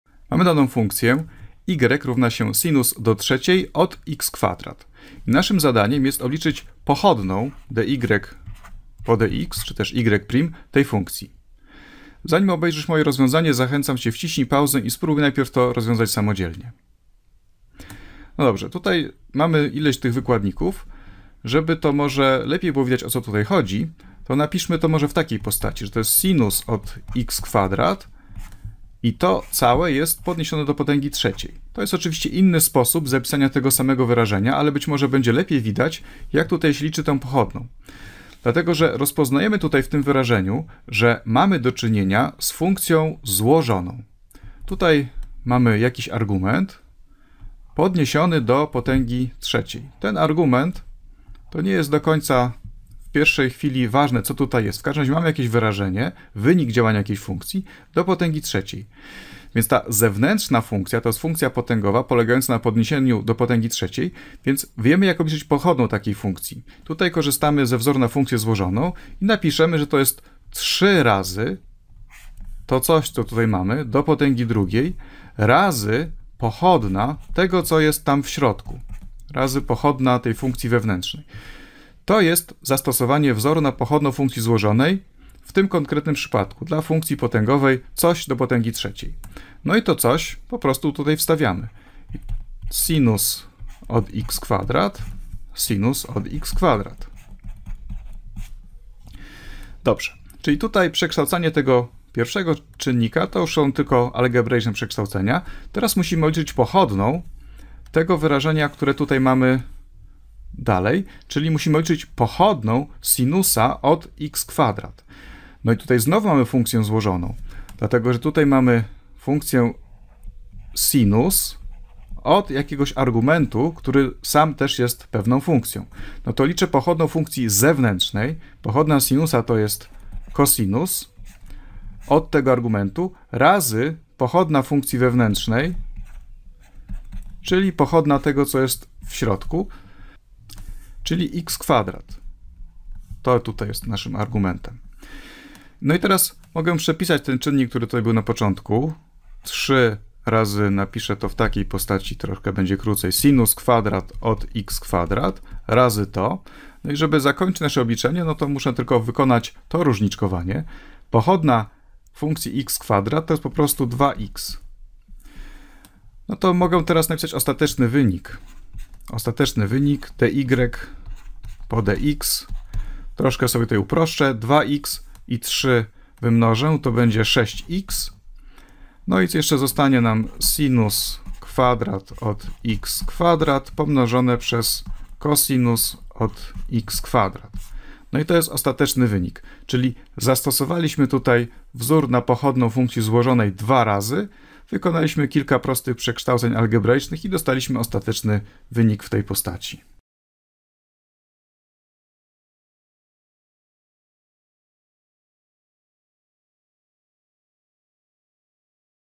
Dwukrotne zastosowanie wzoru na pochodną funkcji złożonej
Film na licencji CC NC-BY-SA zrealizowany przez Fundację Edukacja dla Przyszłości dzięki wsparciu Fundacji Akamai. Opowiada Szymon Charzyński z Wydziału Fizyki Uniwersytetu Warszawskiego.